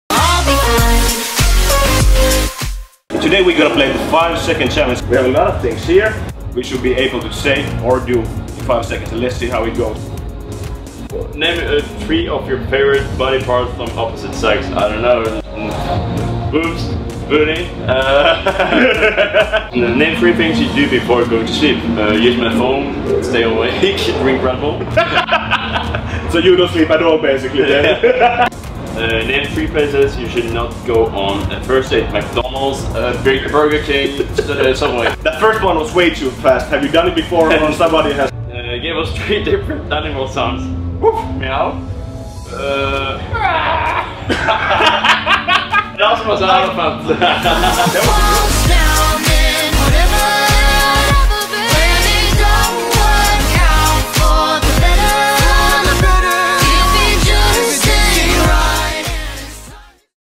알렌 워커의 5초 안에 대답하기 게임!
엉덩이, 레드불, 버거킹, 코끼리까지 상상을 초월하는 답변이 마구마구 튀어나오네요